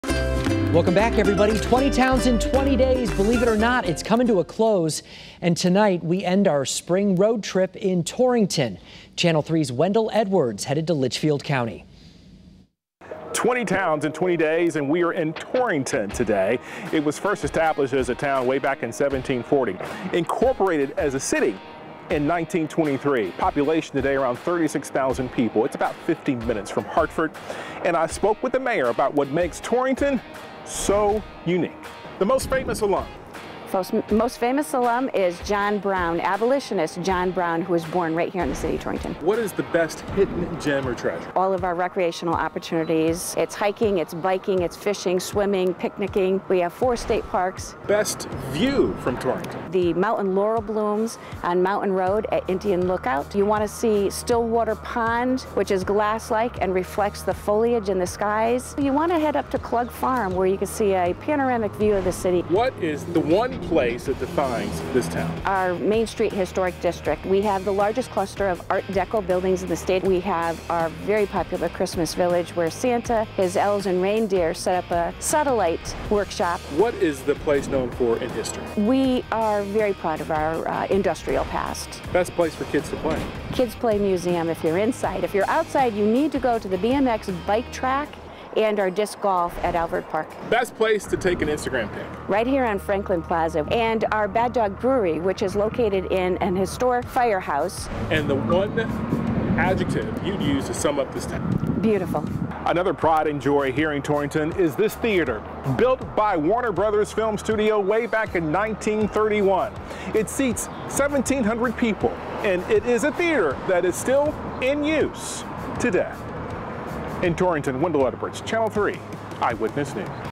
20 Towns in 20 Days: Taking a trip to Torrington
As 20 Towns in 20 Days comes to a close, we are taking a trip to Torrington.